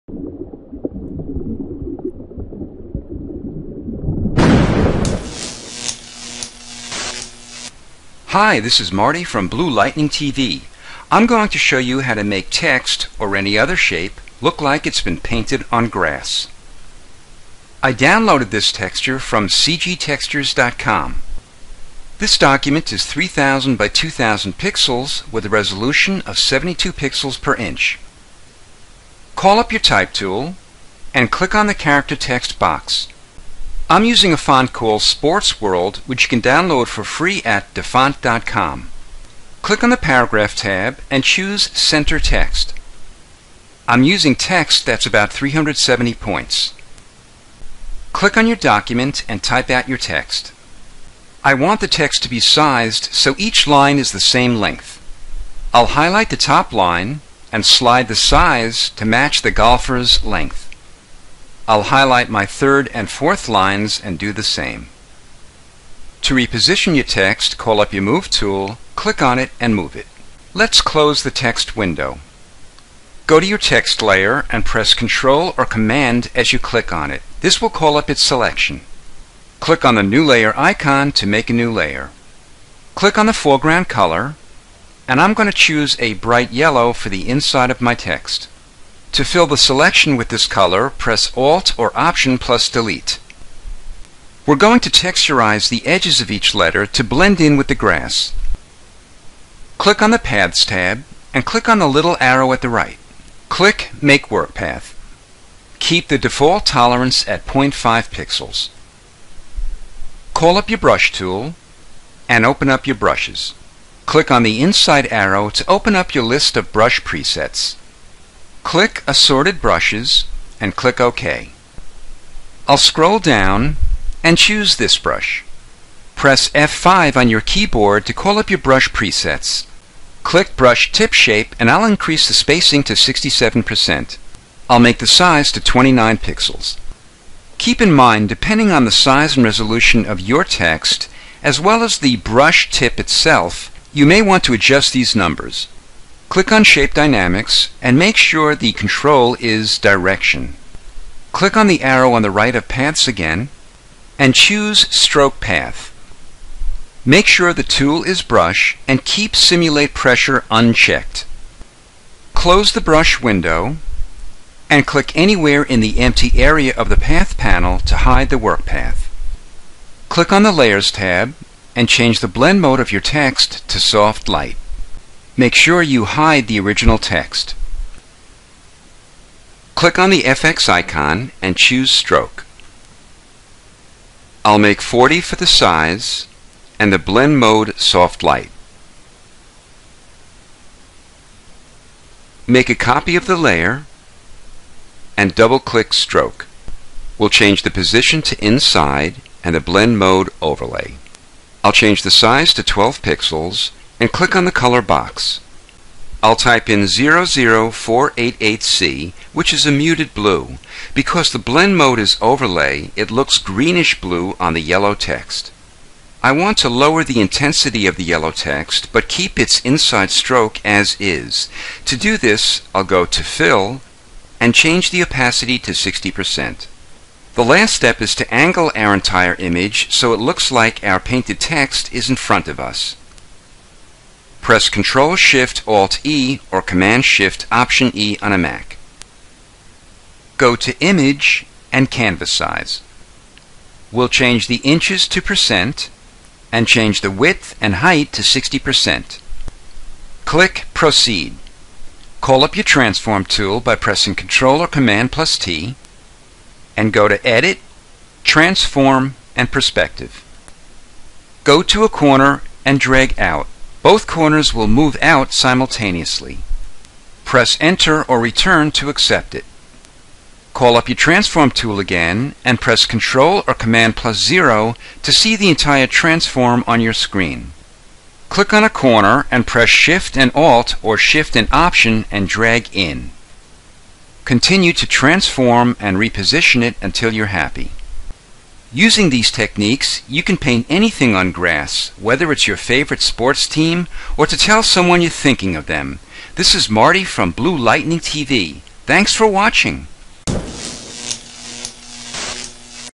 Photoshop Tutorial: GRASS TEXT. How to MakeText look like it's painted on Grass.
Photoshop tutorial showing how to make text, logos or other images look like they're painted on grass.